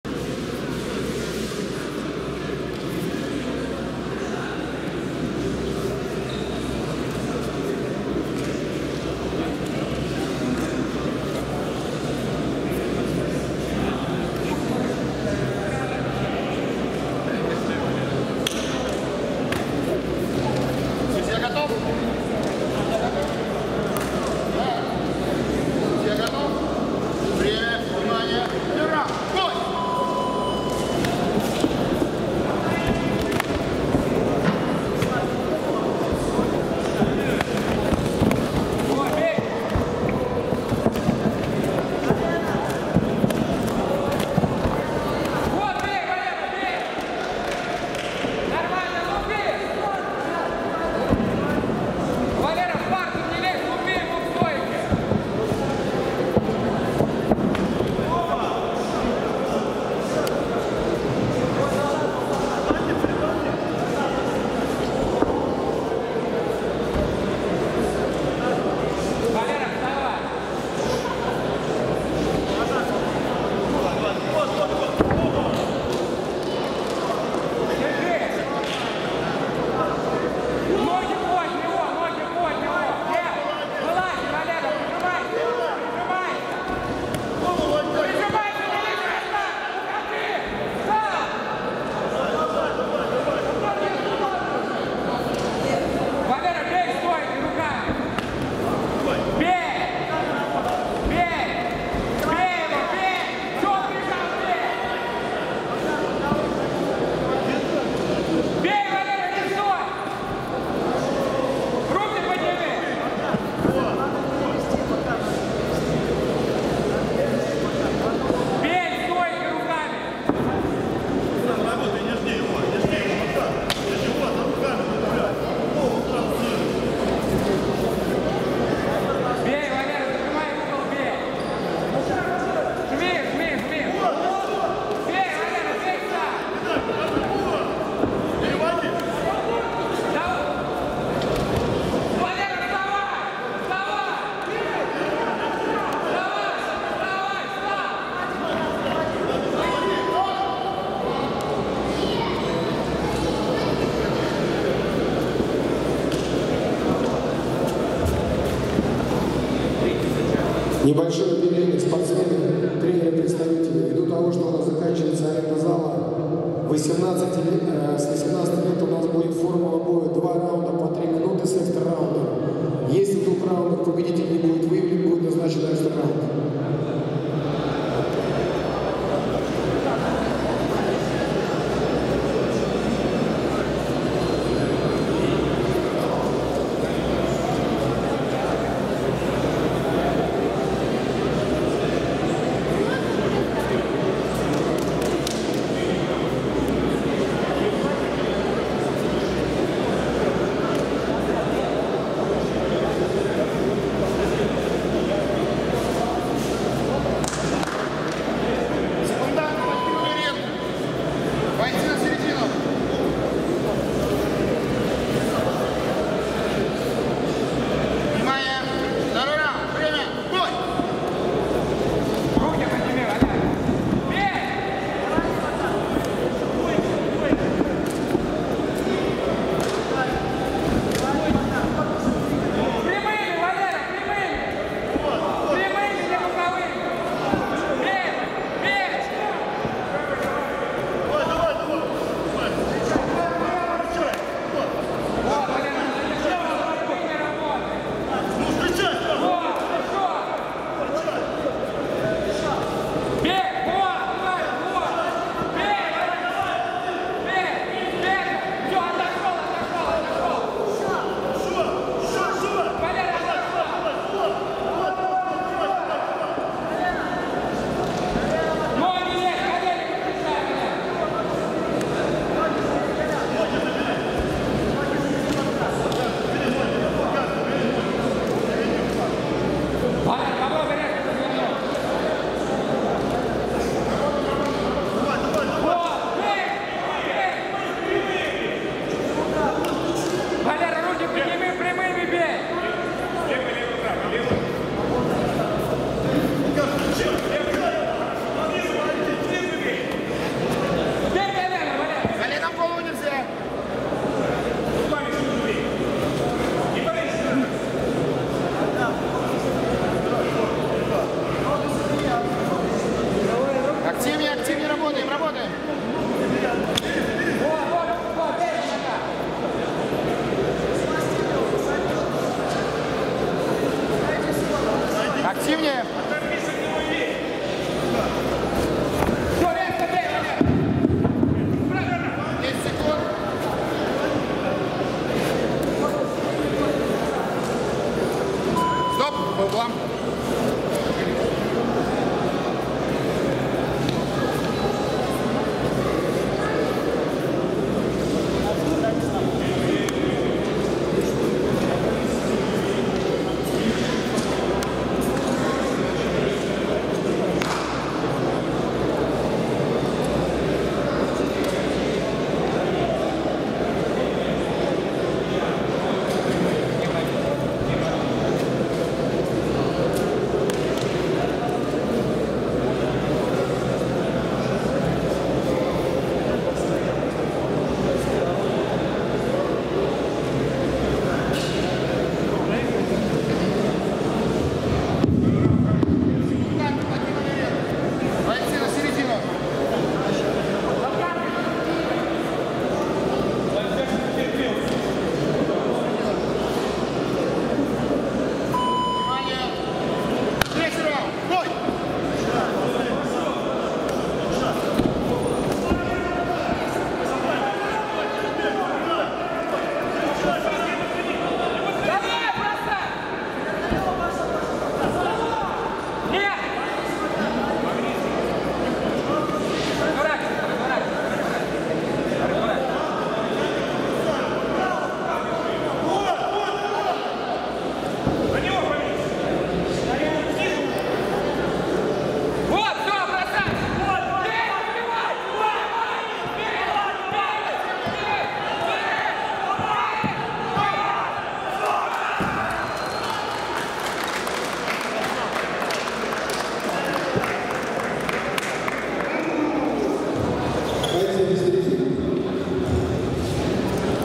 Федоров М (Сафоново) -Титков В (Самбо)
Финал в категории 16-17 лет вес до 120 кг. на Кубке Спартыв Смоленске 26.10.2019г.